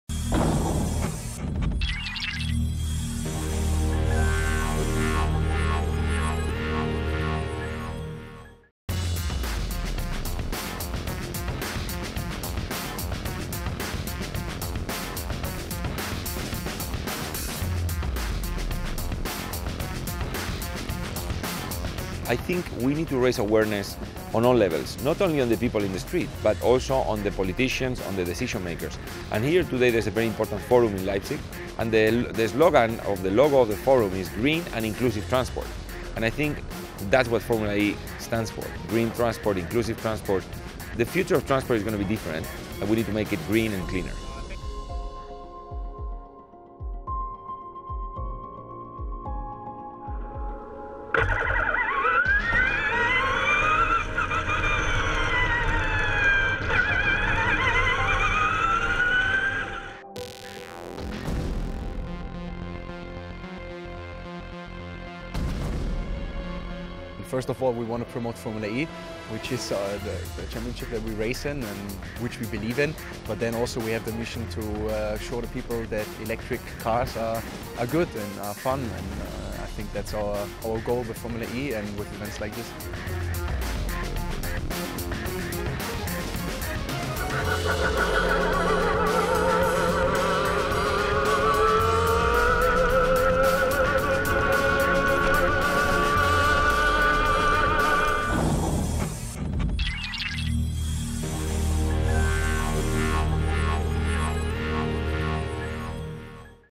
BerlinePrix – Everything starts in Leipzig [Schaeffler]
The FIA Formula E is being presented in the surrounding of the Electric Transport Forum at Leipzig. ABT Schaeffler Audi Sport driver Daniel Abt drove some demo laps to give the assembled politicians a special impression of a electrifying future.